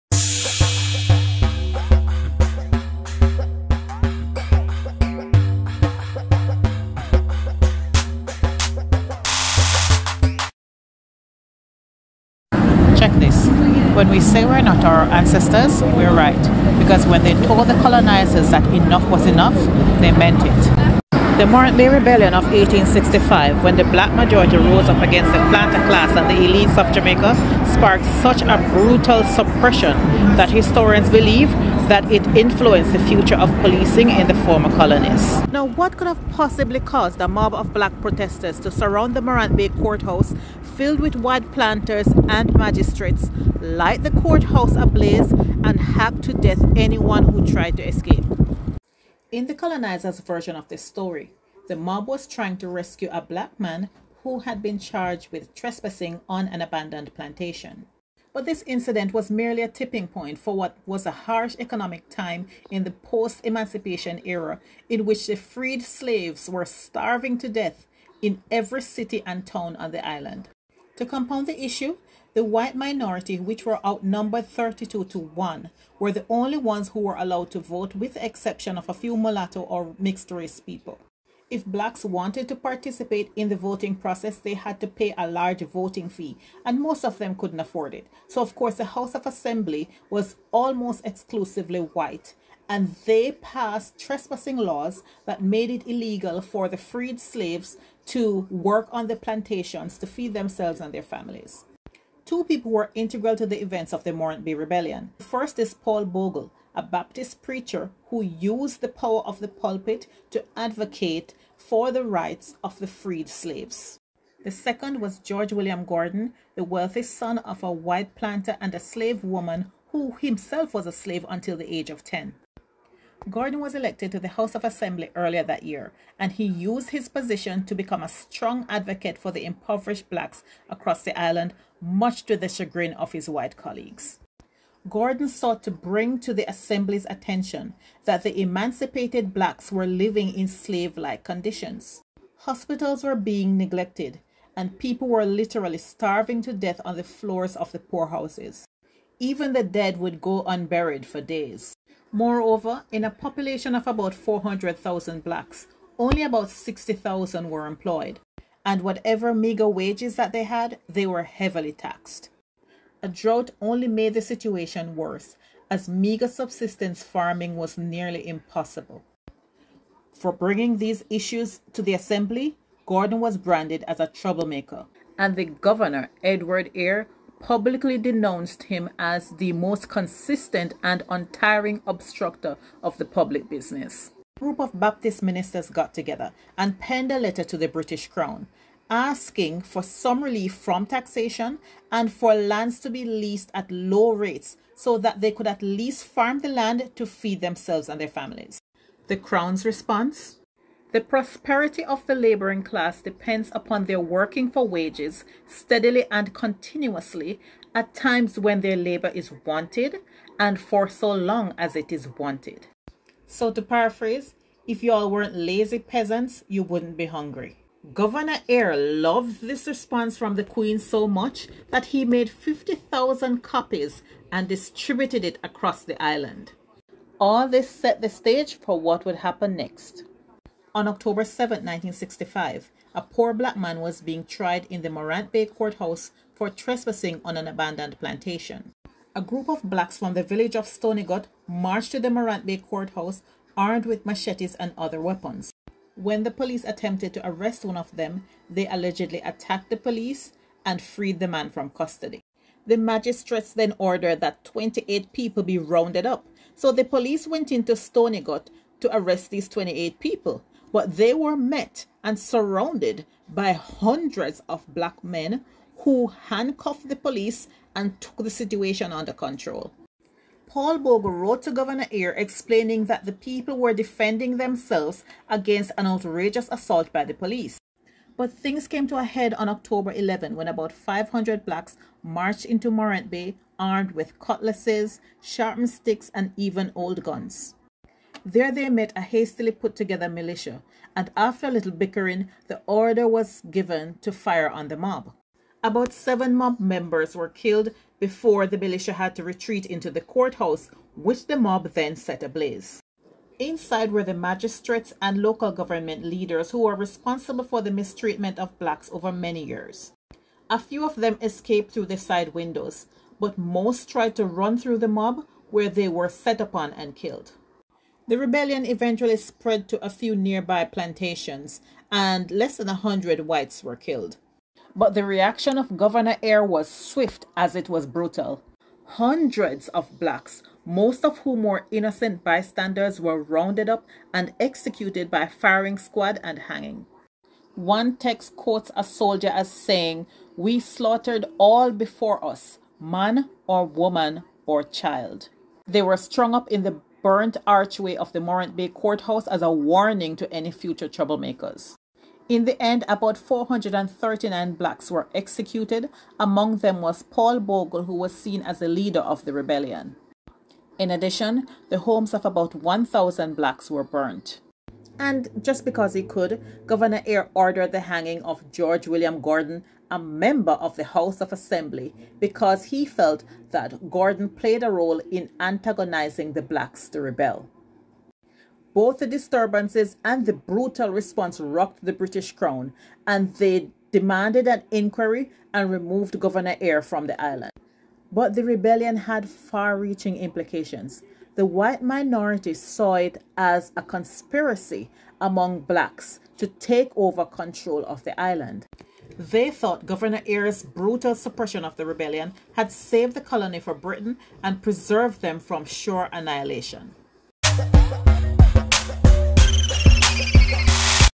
Jamaican Black History - The Black Rebellion of 1865 (Morant Bay)
The Morant Bay Rebellion of 1865 sparked a brutal backlash from the island's governor and may have influenced policing throughout the former colonies.

Sources:
Britannica.com
Encyclopedia.com
UofMiami library online
Historyextra.com
National Library of Jamaica